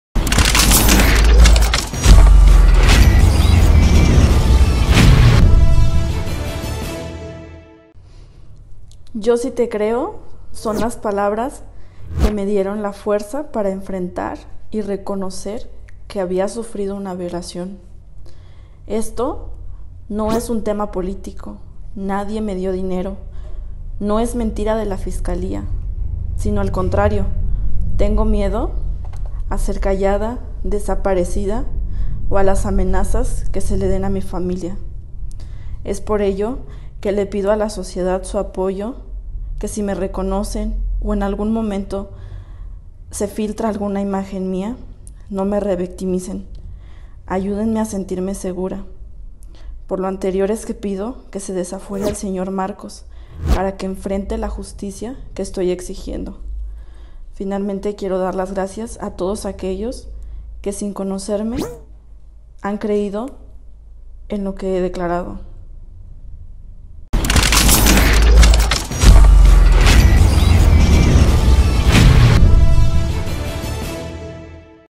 Presunta victima de violación exige desafuero del diputado Marcos Zapotitla y teme ser desaparecida
Presunta victima de violación exige desafuero del diputado local de Morelos Marcos Zapotitla

Esta mañana en conferencia de prensa, abogados de la presunta víctima de violación por parte del diputado Marcos Zapotitla, dieron a conocer que hay presiones y amenazas en contra de la presunta víctima, su familia y el despacho de abogados que la representa.

Los abogados dieron a conocer un video donde la denunciante ofrece unas palabras  a la opinión pública y dice que el caso  no es político, niega haber recibido dinero para denunciar al diputado y exige su desafuero.